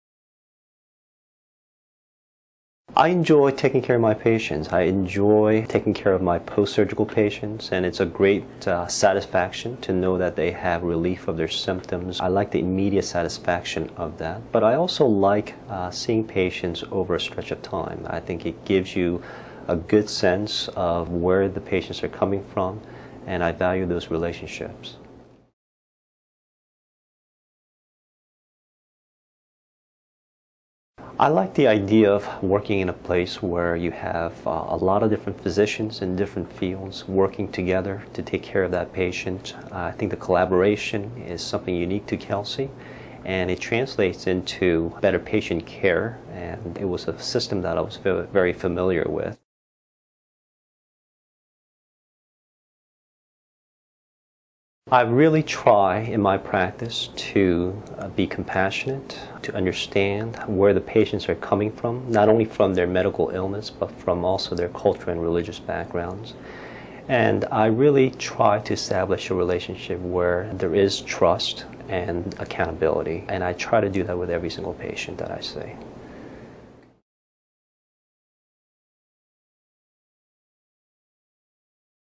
Christopher Chon, MD | Urology | Kelsey-Seybold
Christopher Chon, MD, specializes in Urology and provides care for patients at Kelsey-Seybold Clinic in Houston, Texas.